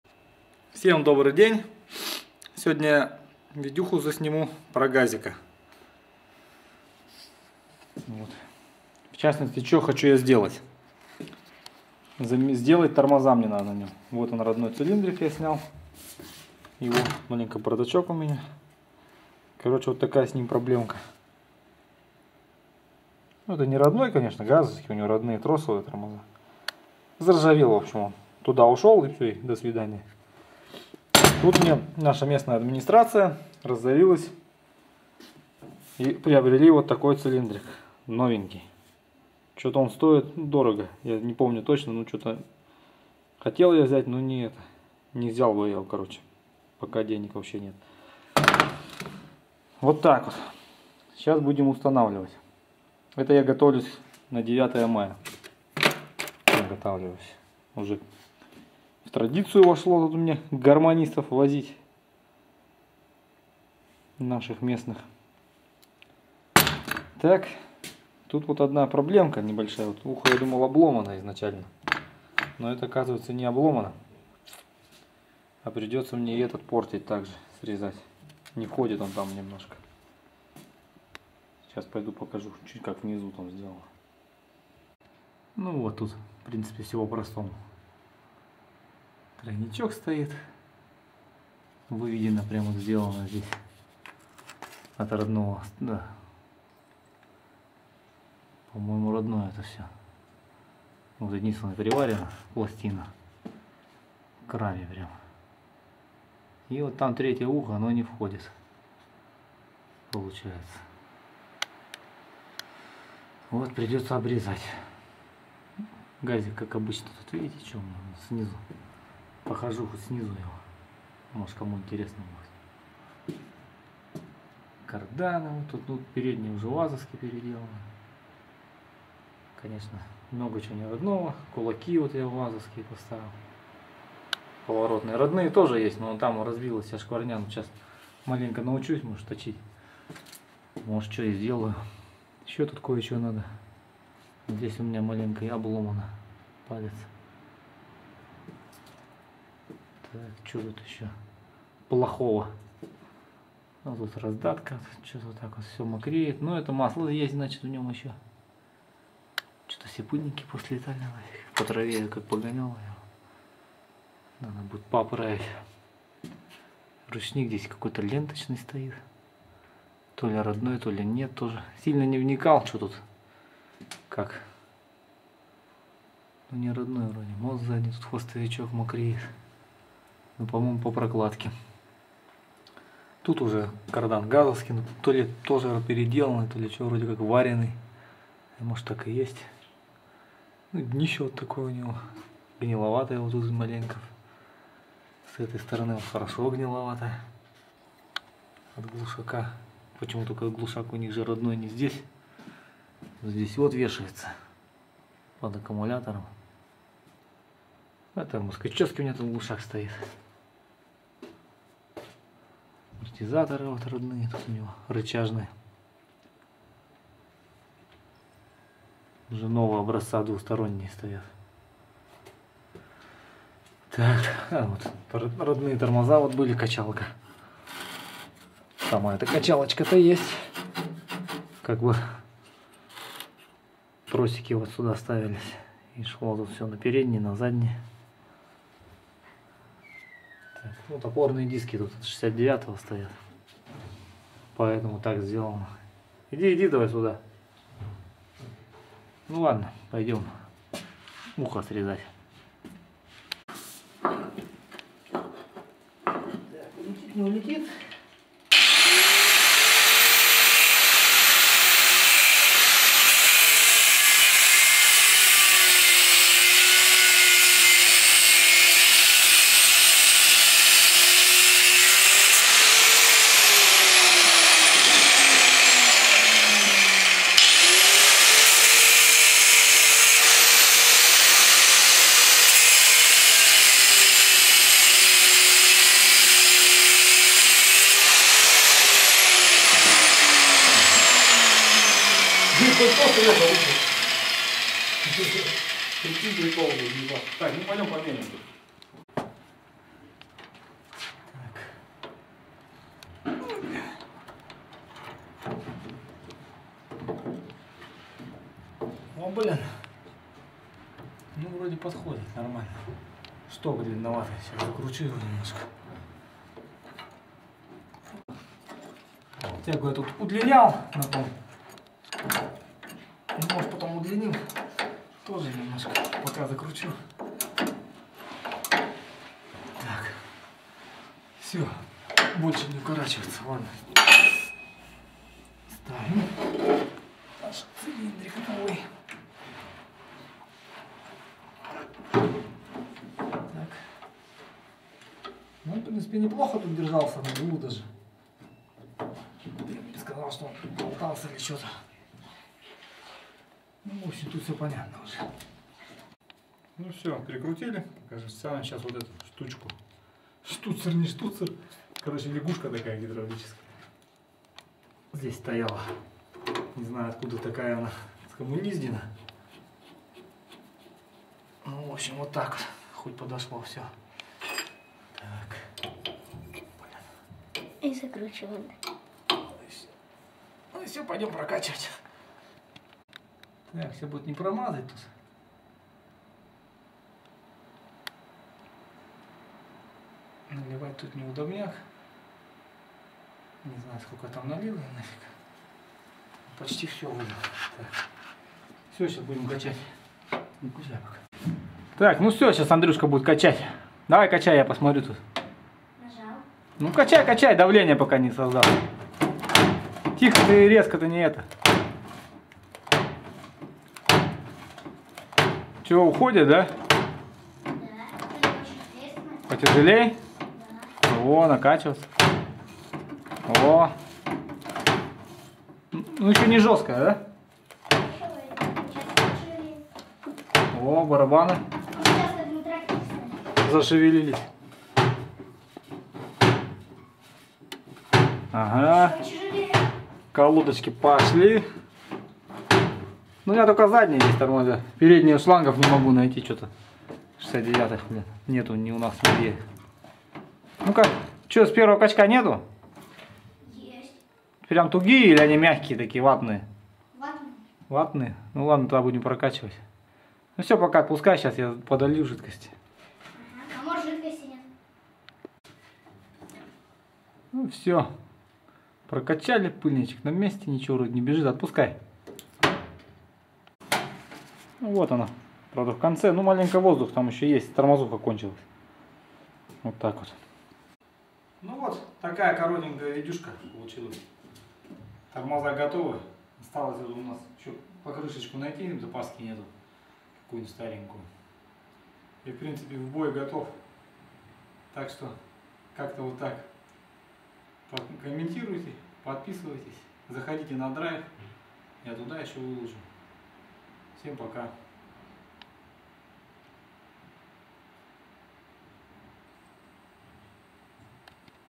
Замена Г.Т.Ц. на ГАЗ 67.
Готовим машину к 9 мая.